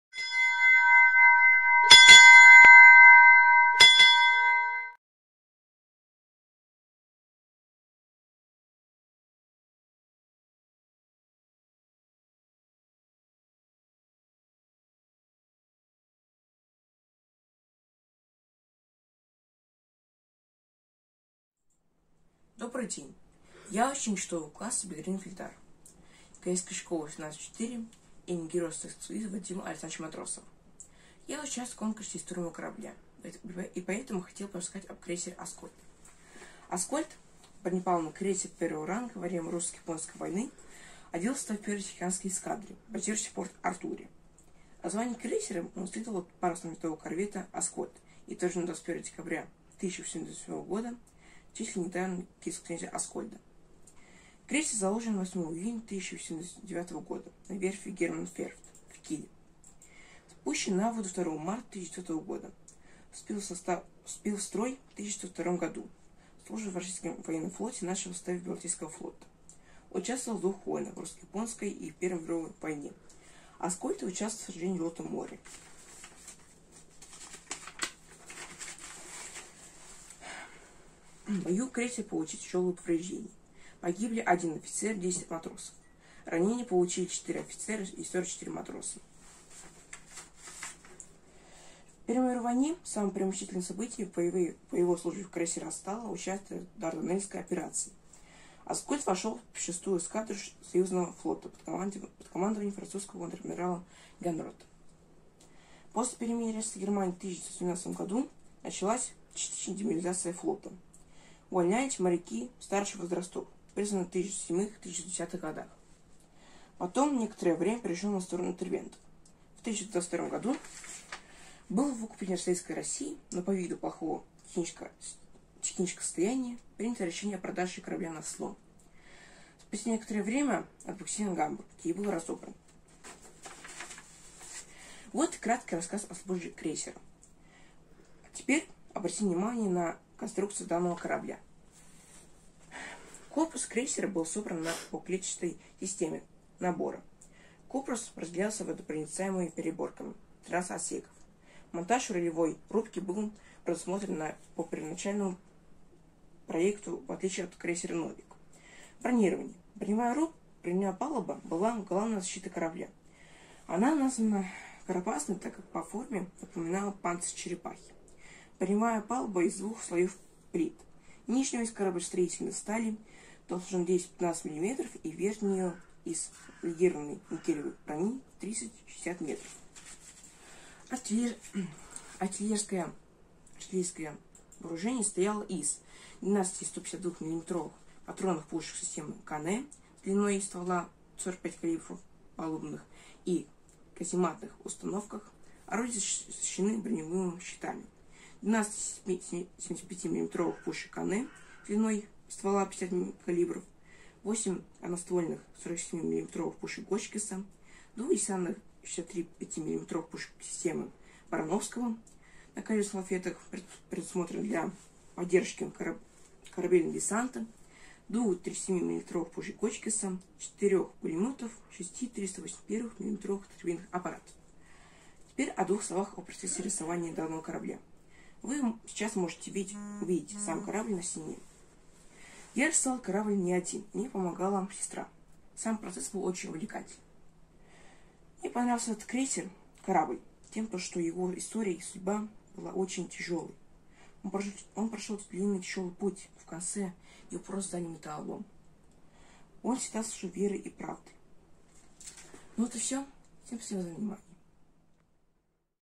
Бедретдинов Ильдар. Номинация "Рисунок"
Голосование за финалистов конкурса «История моего корабля» в номинации «Приз зрительских симпатий»